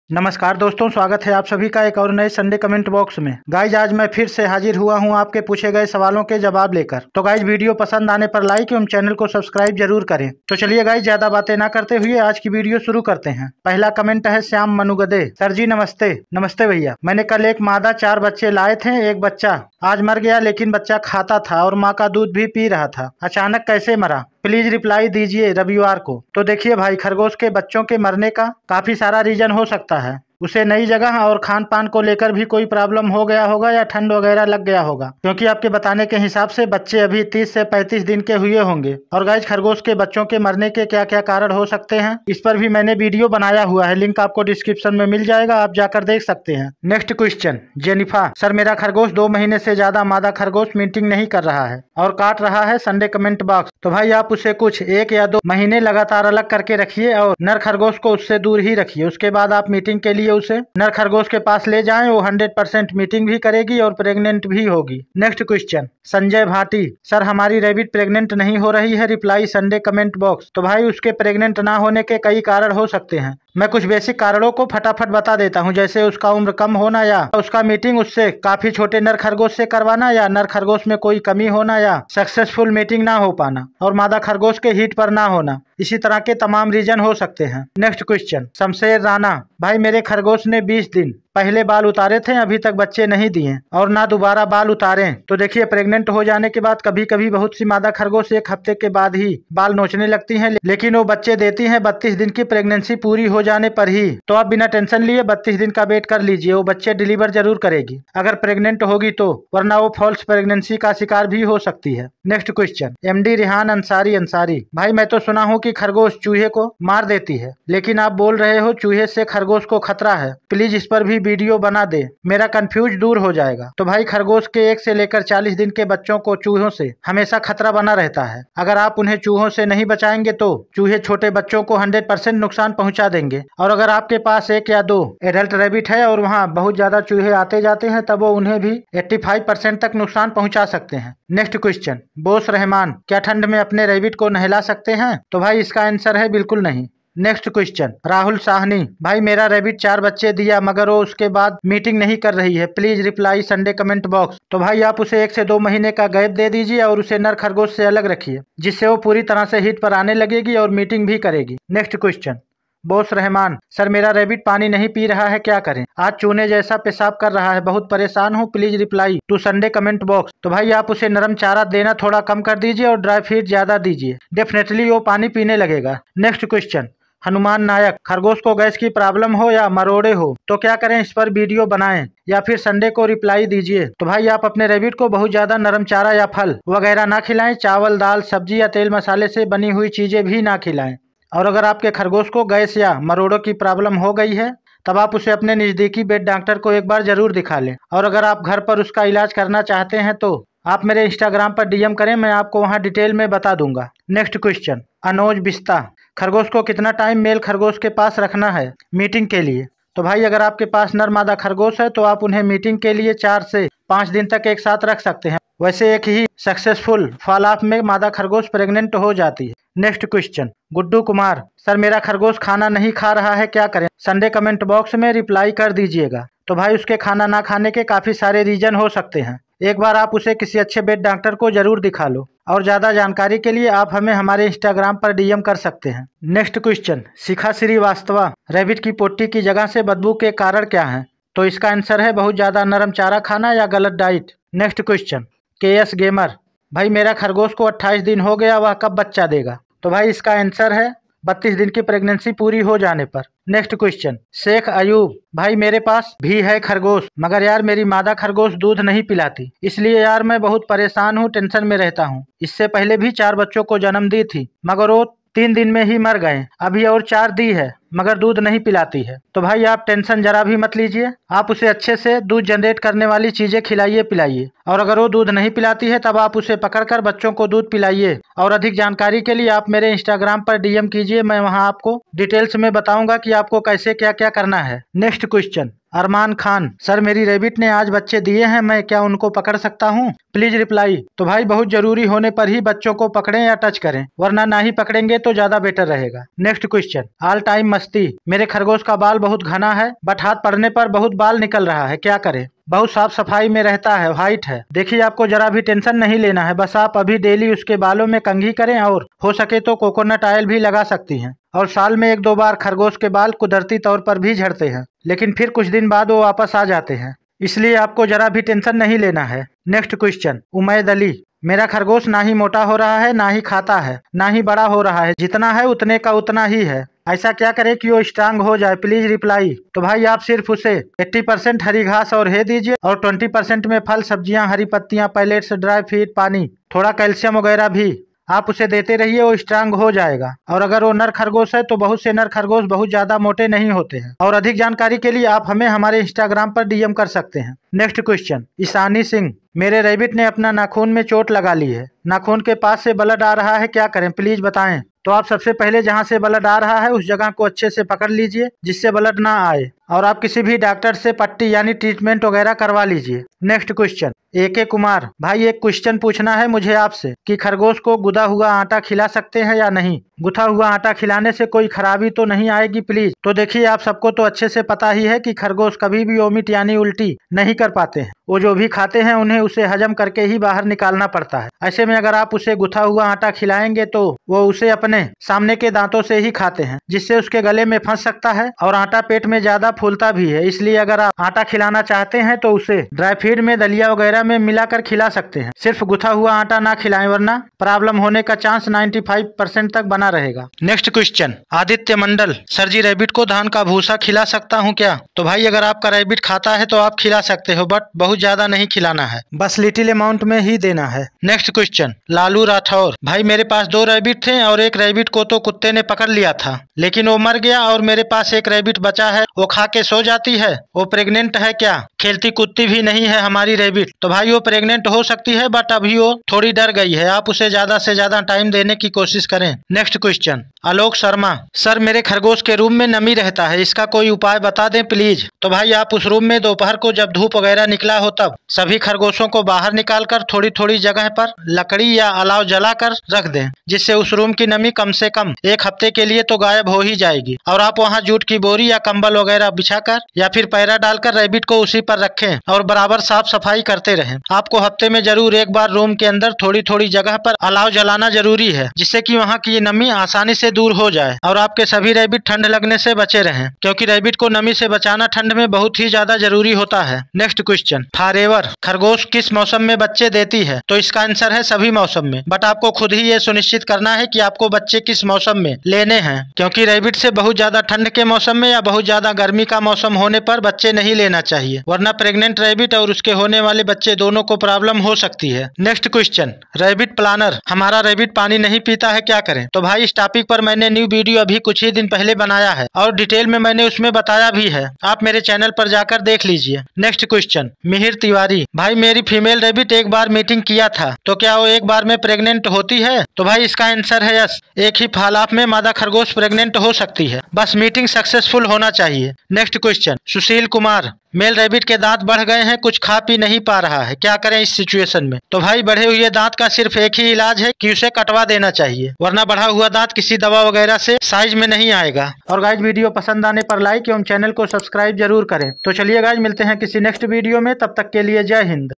क्या खरगोश को गुथा हुआ आटा खिला सकते हैं? क्या खरगोश को धान का भूसा खिला सकते हैं? Sunday Coment Box
नमस्कार दोस्तों स्वागत है आप सभी का हमारे YouTube चैनल पर। दोस्तों इस विडियो में हमनें    आपके पूछे गए सवालों के जवाब सही ढंग से देने की और आपकी हेल्प करने की पूरी कोशिश की है जैसे खरगोश को धान का भूसा खिला सकते हैं या नहीं? खरगोश को घुसा हुआ आटा खिला सकते हैं या नहीं?

some impotent videos link:-